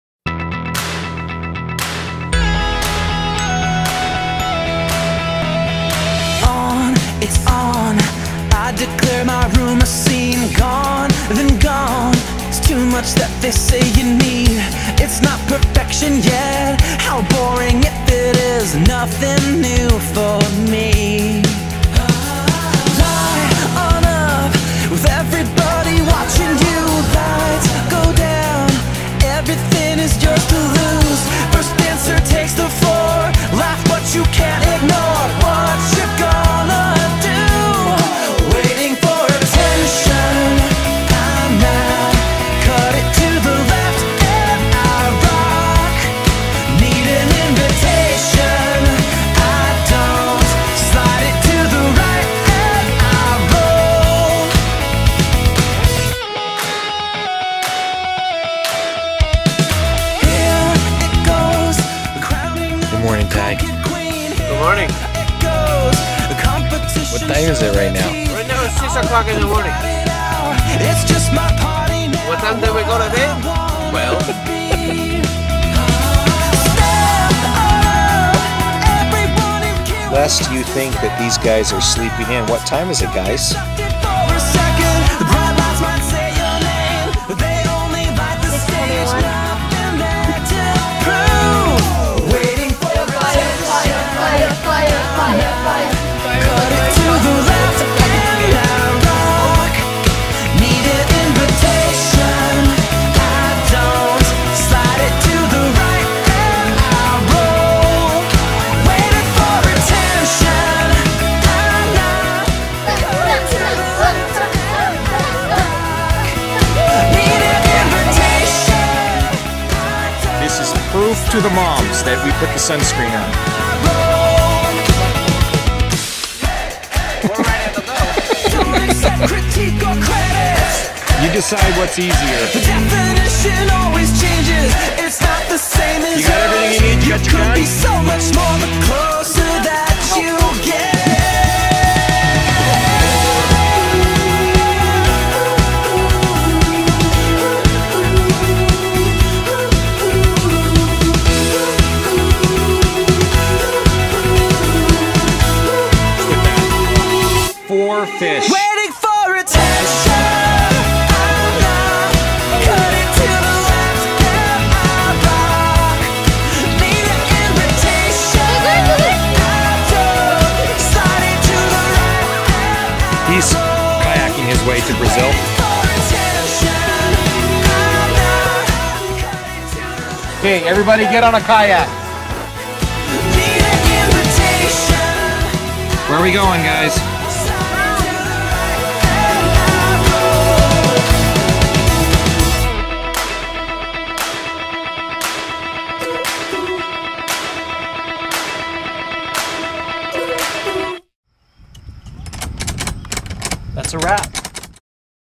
2009.05.22 - Burriston Ponds
After a couple backpacking trips, we decided to relax a little on this trip. We traveled 80 miles south to Burriston Ponds, just outside Mona, Utah. We had an interesting dinner after discovering that the scoutmaster had forgotten several key items, including matches! We were able to borrow matches from neighboring campers, but only after a lecture on "Being Prepared!" The next morning, we enjoyed ourselves Kayaking on the ponds!